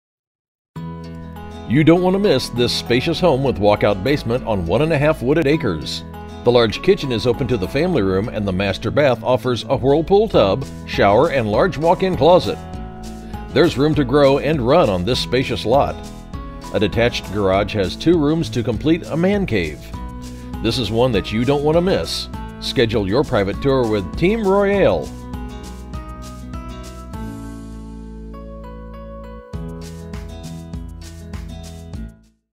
8580 South Providence Drive Nineveh, IN | MLS# 21546795
8580 South Providence Drive, Nineveh, IN

Spacious home with walk-out basement on 1.5 wooded acres + 2 garages. Large kitchen is open to family room, master bath has whirlpool tub + shower-large walk-in closet too! Room to grow and run on this spacious lot and 2 garages for hobbies or needs. Extra room in basement could be office or guest room. New roof and siding. Detached garage has two rooms to complete man cave!  Just reduced - Bring offer!